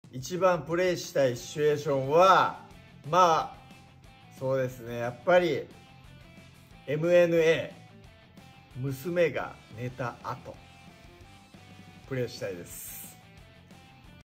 DAIGOのディアデイズをプレイしたいシチュエーションは？
ディアデイズCM撮影後のDAIGOさんに突撃！

「Nintendo Switch™・Steam®︎用ソフト
「カードファイト!! ヴァンガード ディアデイズ」
ブシロードによる超大作オリジナルカードゲーム『カードファイト!! ヴァンガード』のoverDressシリーズが遊べる初のデジタル商品である「カードファイト!! ヴァンガード ディアデイズ」のオープニングストーリーを公開！
Nintendo Switch™パッケージ版予約受付中！
初回生産分には【特製PRカード(3枚)】封入！